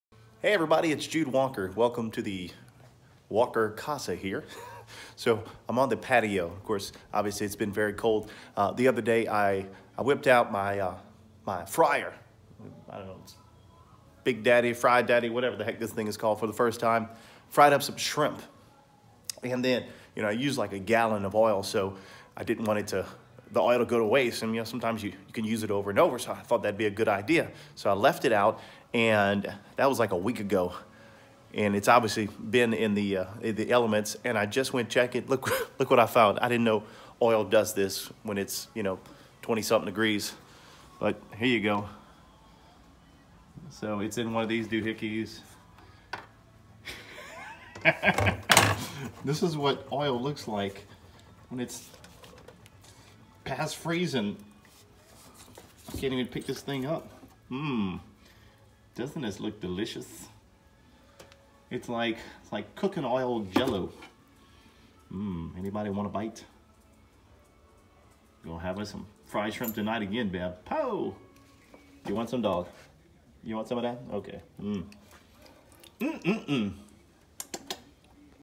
What does frozen cooking oil look like?
Jude Walker shows you what cooking oil looks like after it’s been left outside in sub-freezing temperatures.